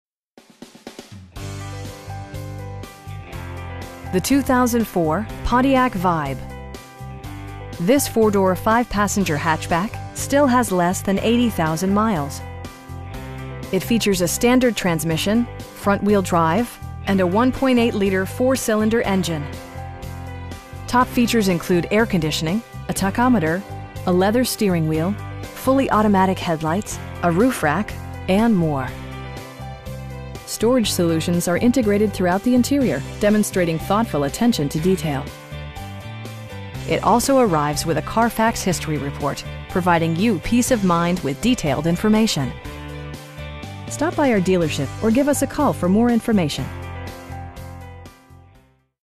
2004 Pontiac Vibe GT in Rome, NY 13440
Rome Pre-Owned Auto Sales

The 2004 Pontiac Vibe. This 4 door, 5 passenger hatchback still has less than 80,000 miles! It features a standard transmission, front-wheel drive, and a 1.8 liter 4 cylinder engine. Top features include air conditioning, a tachometer, a leather steering wheel, front bucket seats, fully automatic headlights, power door mirrors, a roof rack, and more. Storage solutions are integrated throughout the interior, demonstrating thoughtful attention to detail. It also arrives with a Carfax history report, providing you peace of mind with detailed information. We have the vehicle you've been searching for at a price you can afford. Stop by our dealership or give us a call for more information.